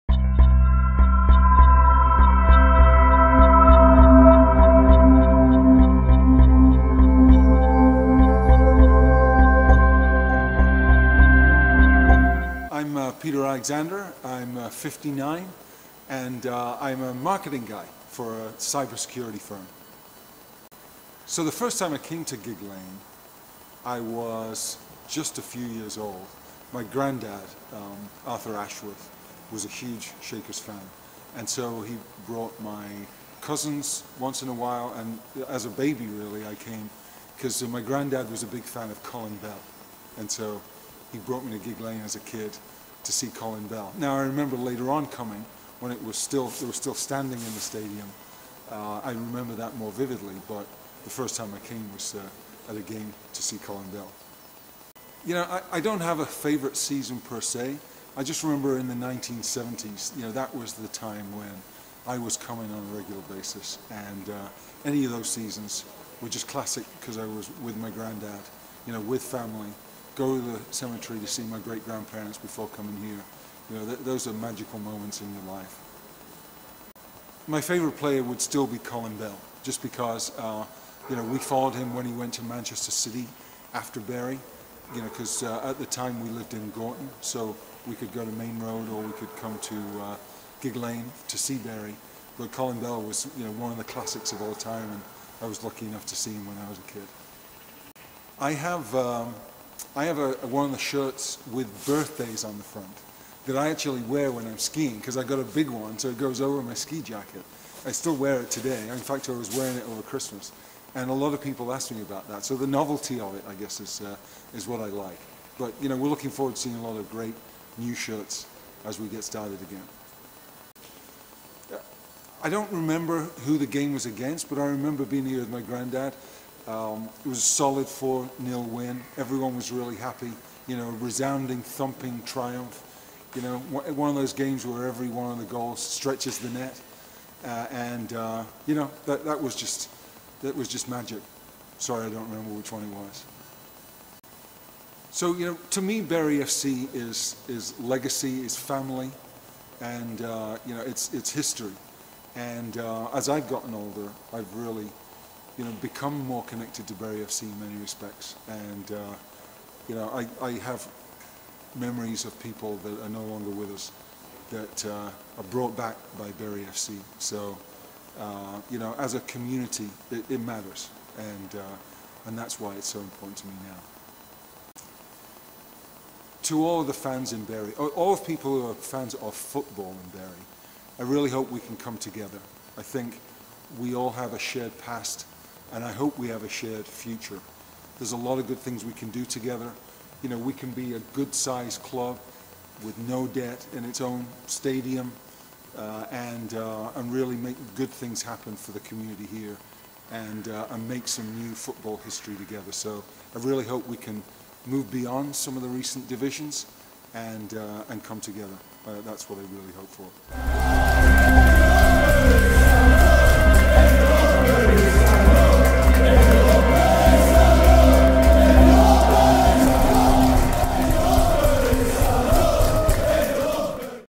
Meet Peter Alexander
Peter Alexander is one of the benefactors that makes up 49% of the ownership model. A Shakers fan now based in USA, Peter’s relationship with the club stretches back generations. Here we get to know a little more about his connection with the club.